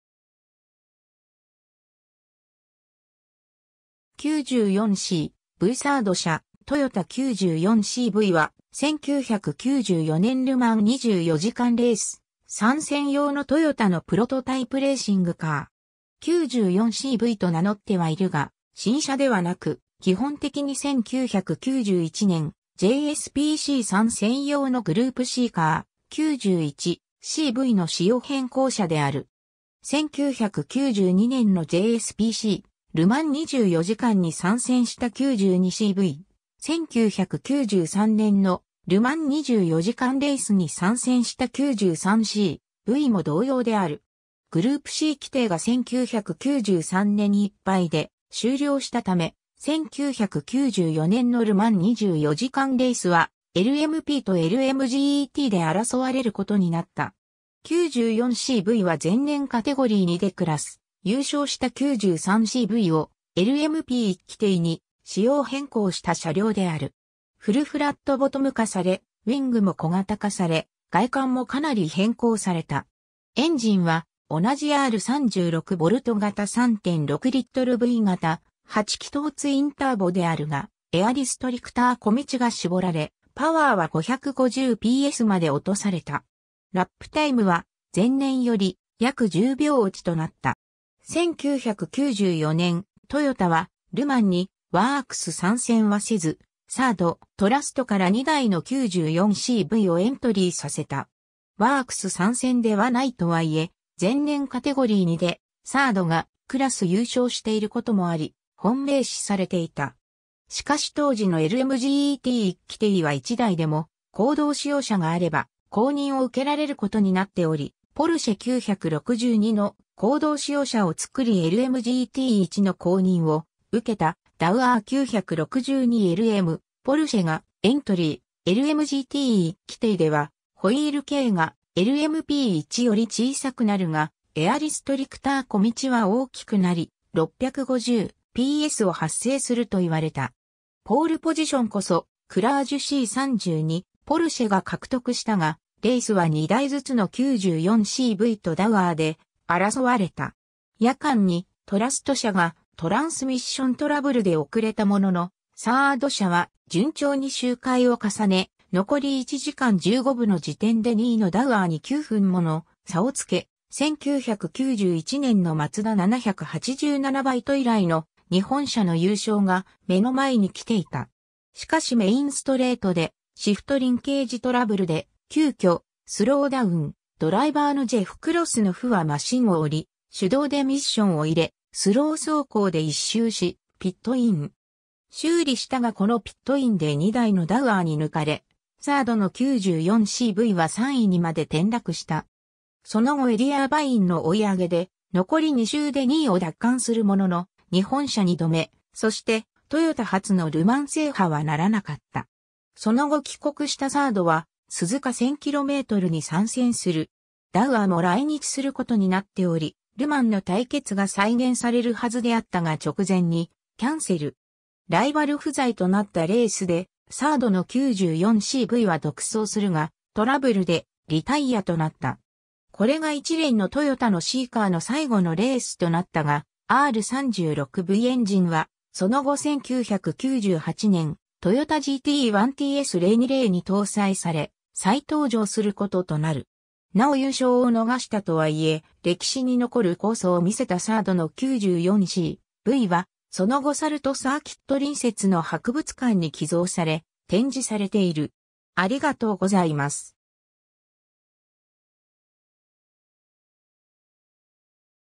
トヨタ・94C-V
94C-V サード車 トヨタ・94C-V (Toyota 94C-V) は、1994年ル・マン24時間レース参戦用のトヨタのプロトタイプレーシングカー。
 94C-Vと名乗ってはいるが、新車ではなく、基本的に1991年JSPC参戦用のグループCカー、91C-Vの仕様変更車である。
1992年のJSPC、ル・マン24時間に参戦した92C-V、1993年のル・マン24時間レースに参戦した93C-Vも同様である。
 グループC規定が1993年一杯で終了したため、1994年のル・マン24時間レースはLMP（ルマンプロトタイプ）とLMGTで争われることになった。
94C-Vは前年カテゴリー2（1990年までのグループC）でクラス優勝した93C-Vを、LMP1規定に仕様変更した車両である。
フルフラットボトム化され、ウィングも小型化され、外観もかなり変更された。
エンジンは同じR36V型 3.6リットル V型8気筒 ツインターボであるが、エアリストリクター径が絞られ、パワーは550PSまで落とされた。
LAPタイムは前年より約10秒落ちとなった。
 1994年、トヨタはル・マンにワークス参戦はせず、サード、トラストから2台の94C-Vをエントリーさせた。
ワークス参戦ではないとは言え、前年カテゴリー2でサードがクラス優勝していることもあり、本命視されていた。
しかし当時のLMGT1規定は1台でも公道仕様車があれば公認を受けられることになっており、ポルシェ・962の公道仕様車を作りLMGT1の公認を受けたダウアー・962LM/ポルシェがエントリー、LMGT1規定ではホイール径がLMP1より小さくなるが、エアリストリクター径は大きくなり、650PSを発生すると言われた。
ポールポジションこそクラージュC32/ポルシェが獲得したが、レースは2台ずつの94C-Vとダウアーで争われた。
 夜間にトラスト車がトランスミッショントラブルで遅れたものの、サード車は順調に周回を重ね、残り1時間15分の時点で2位のダウアーに9分もの差をつけ、1991年のマツダ787B以来の日本車の優勝が目の前に来ていた。
しかしメインストレートでシフトリンケージトラブルで急遽スローダウン、ドライバーのジェフ・クロスノフはマシンを降り、手動でミッションを入れ、スロー走行で1周しピットイン。
修理したがこのピットインで2台のダウアーに抜かれサードの94C-Vは3位にまで転落した。
その後エディ・アーバインの追い上げで、残り2周で2位を奪還するものの、日本車2度目、そしてトヨタ初のル・マン制覇は成らなかった。
 その後帰国したサードは、鈴鹿1000kmに参戦する。
ダウアーも来日することになっており、ル・マンの対決が再現されるはずであったが直前にキャンセル。
ライバル不在となったレースでサードの94C-Vは独走するが、トラブルでリタイヤとなった。
これが一連のトヨタのCカー（LMP1規定に改造されていたが）の最後のレースとなったが、R36Vエンジンはその後1998年、トヨタ・GT-One TS020に搭載され再登場することとなる。
 なお優勝を逃したとは言え、歴史に残る好走を見せたサードの94C-Vはその後サルト・サーキット隣接の博物館に寄贈され展示されている。